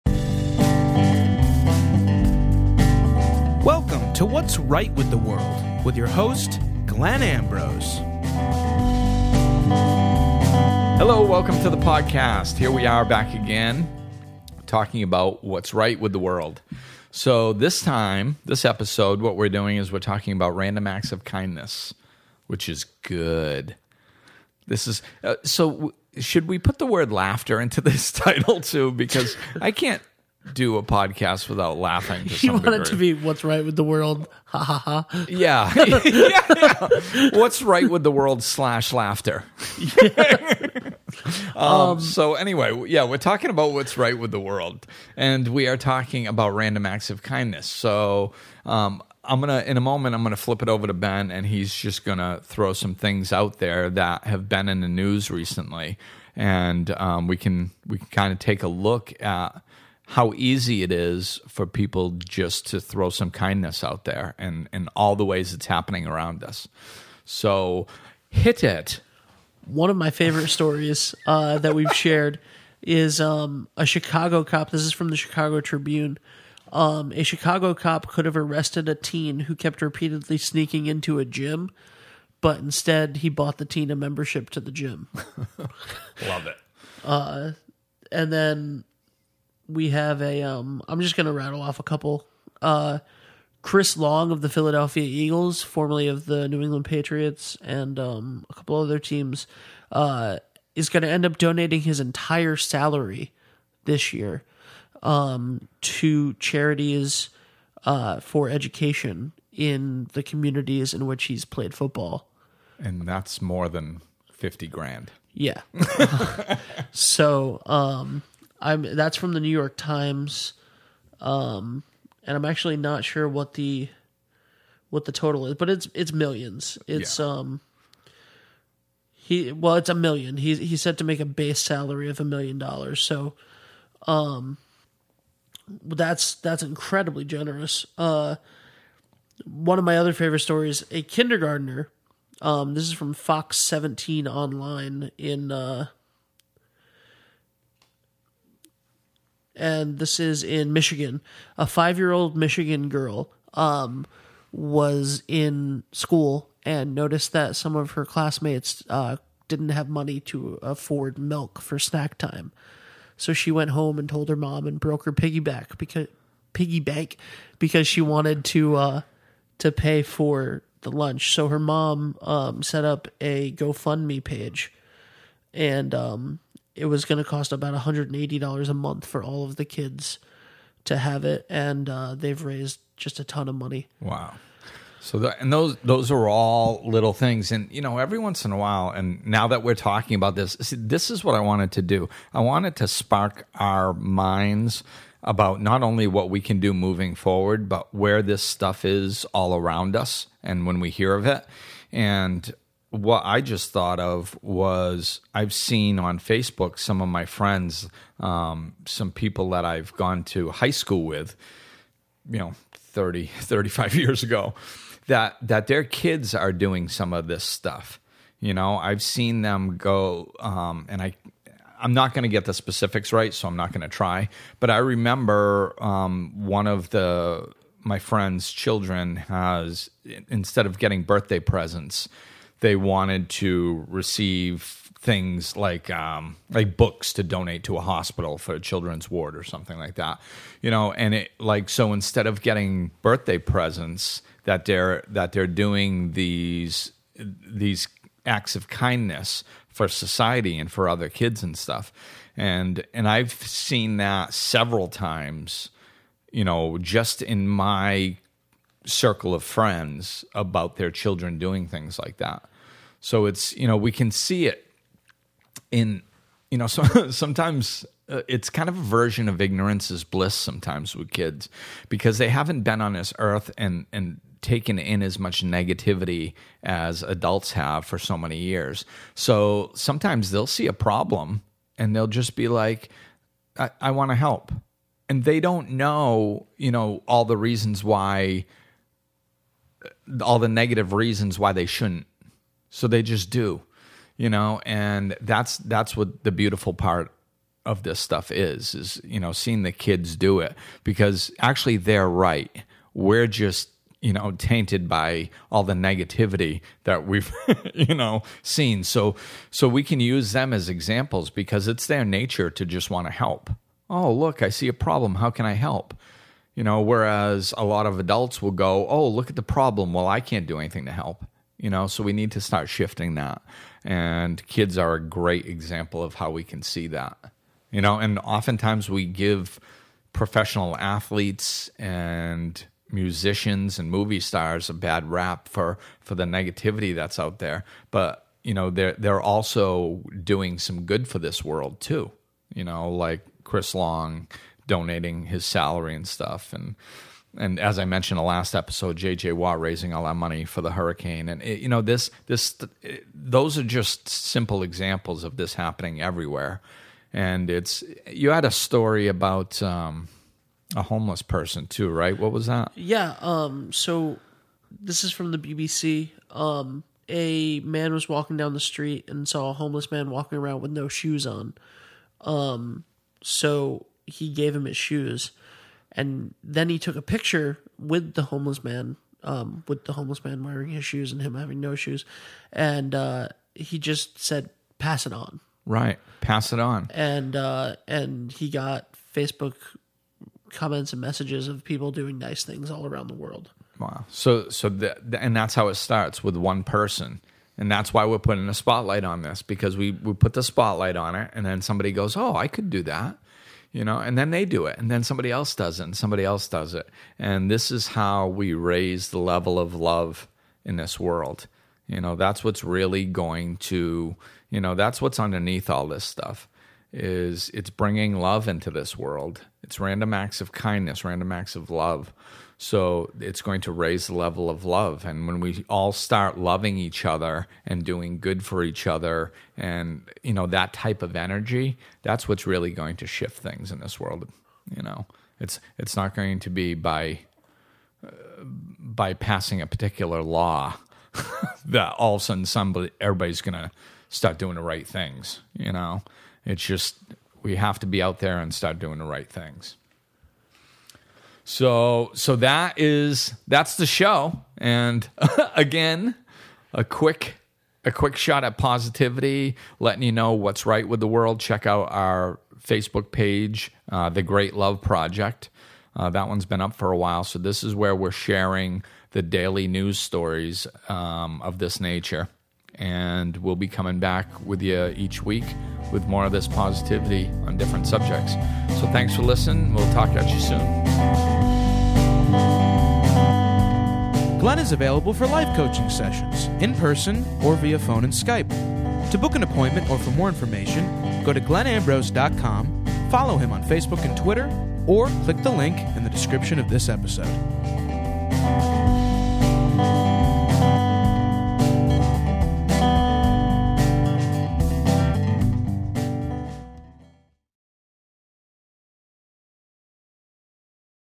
12/6/17: Random Acts of Kindness
In this episode we discuss several news articles of people helping out strangers!

Glenn is available for life coaching sessions in person, or via phone and Skype.